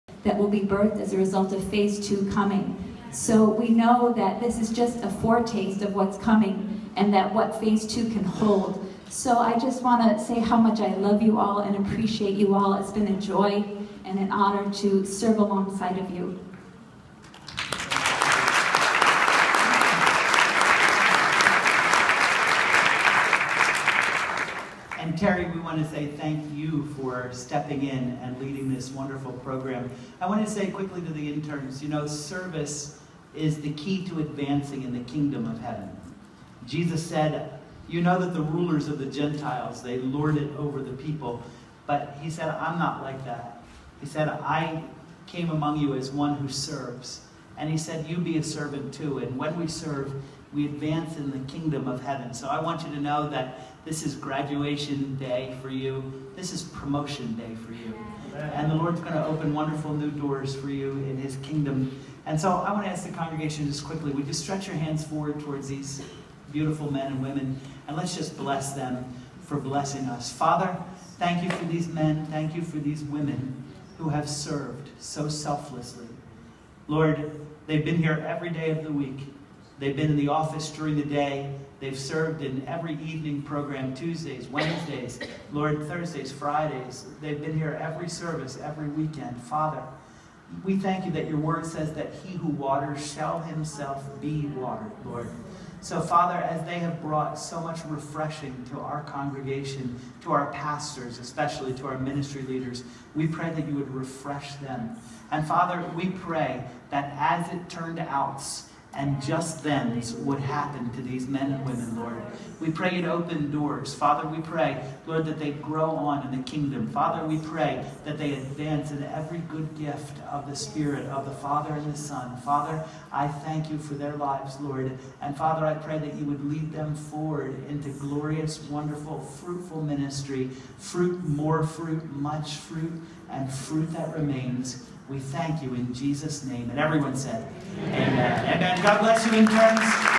Ministry Interns Graduation
Pastor Glenn and Terri Kelly congratulate our first class of Ministry Interns, June 15, 2014